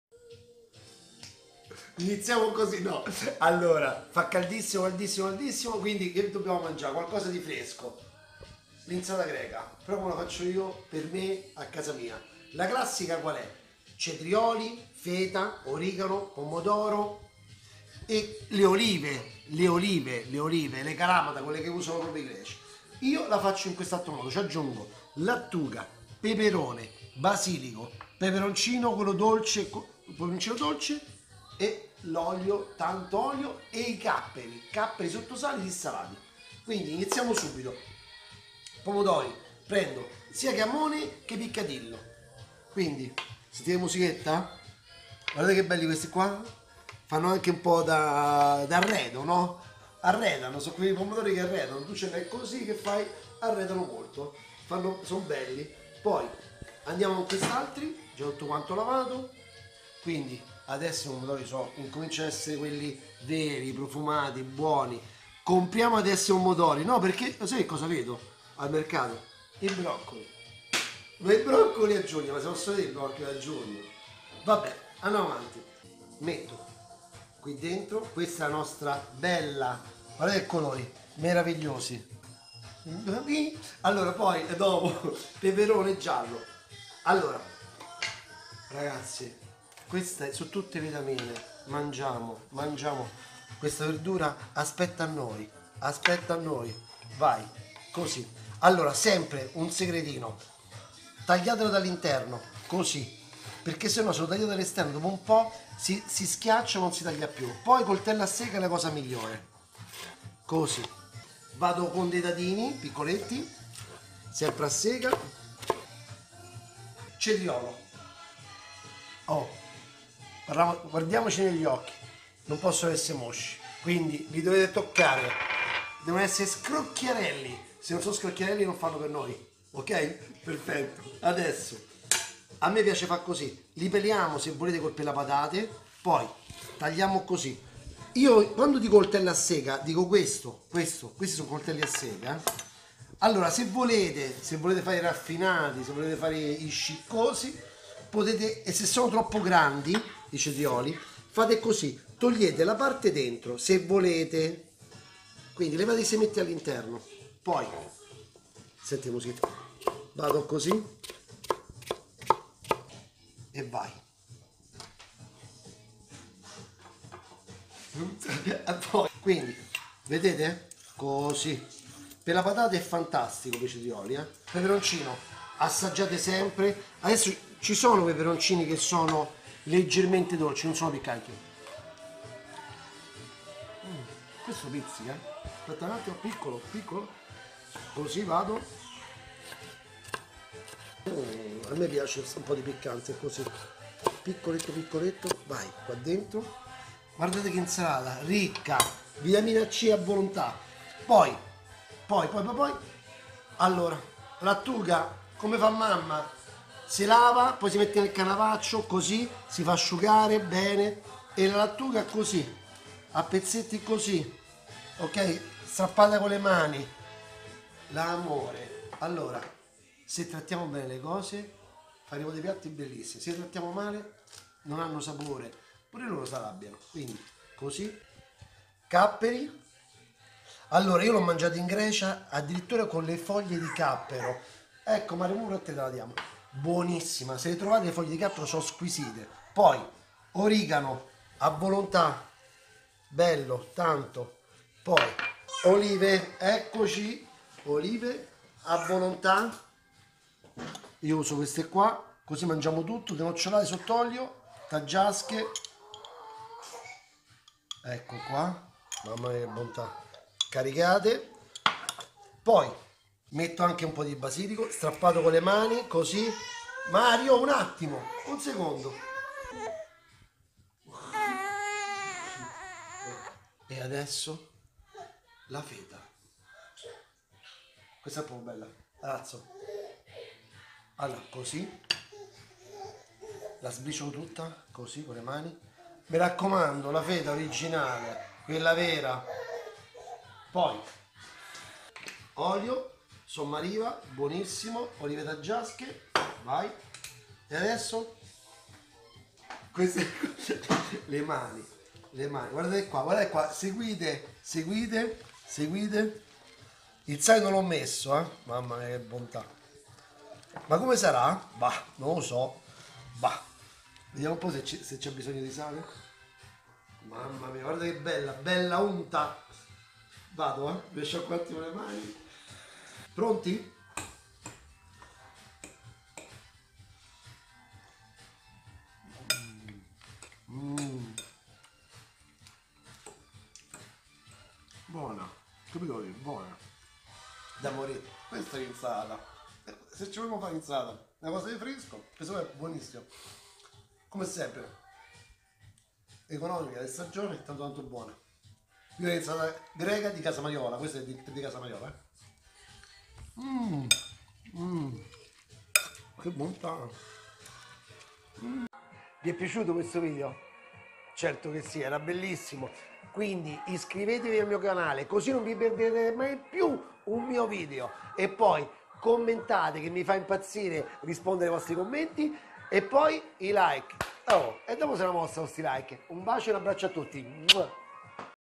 INSALATA GRECA di Casa Mariola - TUTORIAL - la video ricetta di Chef Max Mariola
INGREDIENTI x 2 persone:
formaggio feta
cetrioli
origano
pomodori
olive denociolate
cipolla (opzionale)
olio extravergine di oliva
lattuga
peperone
basilico
capperi
peperoncino dolce

SPECIAL THANKS
Olive e olio extravergine d'oliva
di Sommariva Tradizione Agricola

CUCINA PROGETTATA DA Architetto Pitro Renzi

▼ LISTA AMAZON▼